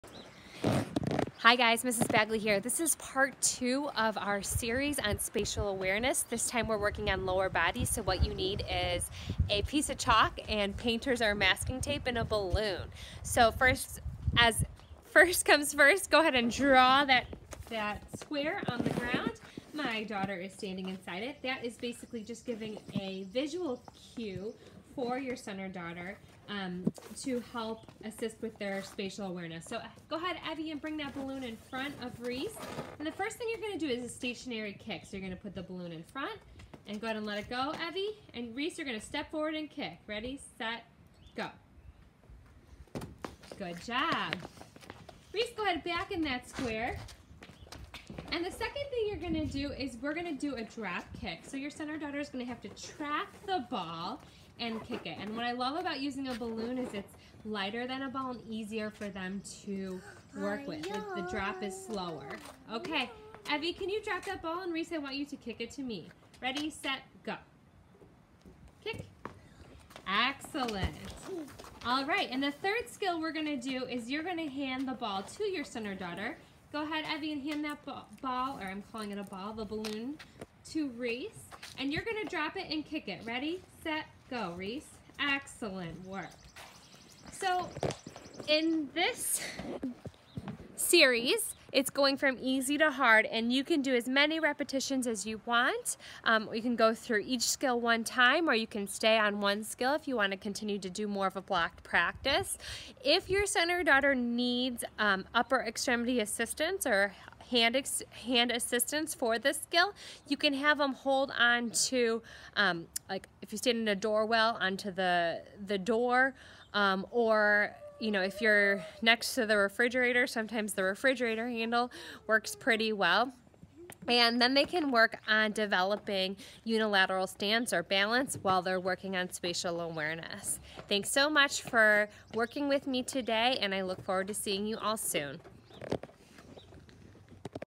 Pediatric PT: Lower extremity body awareness
Working with a balloon to increase lower extremity awareness.